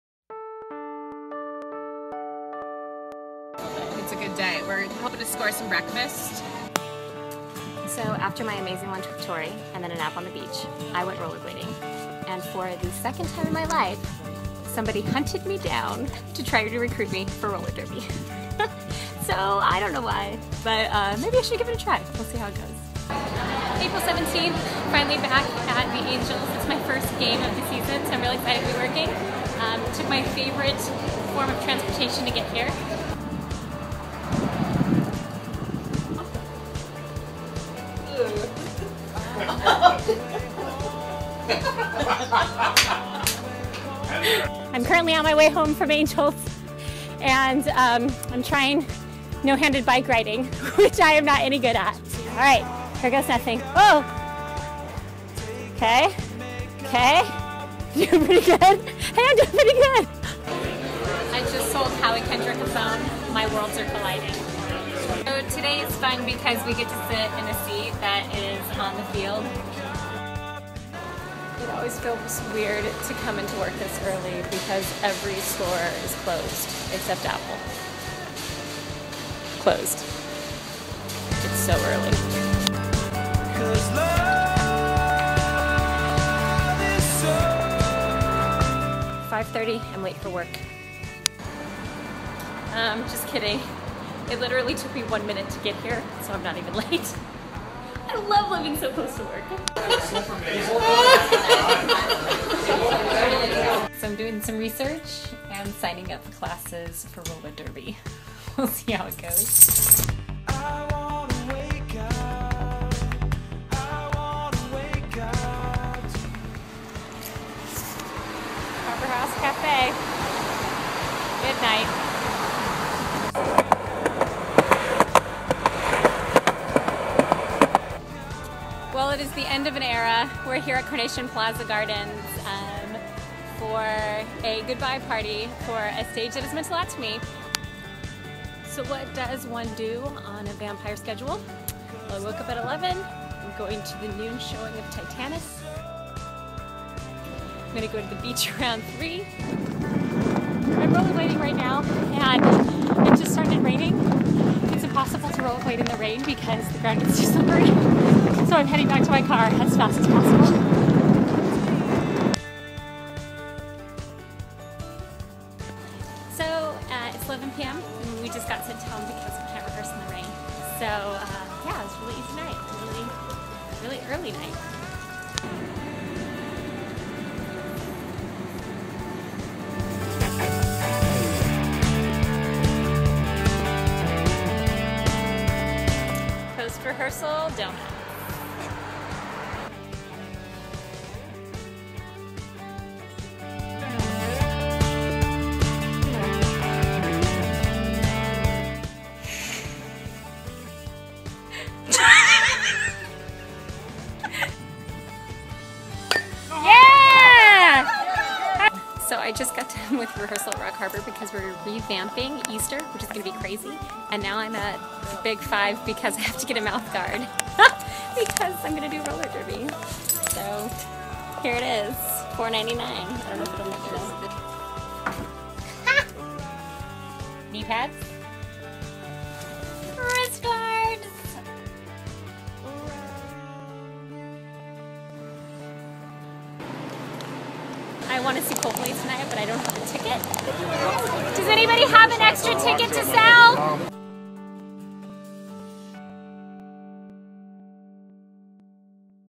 TSLOLH: Episode Two
The Secret Life of Liz Hetzel: Episode 2.  Angels.  Overnight Rehearsals.  Goodbye Carnation Plaza.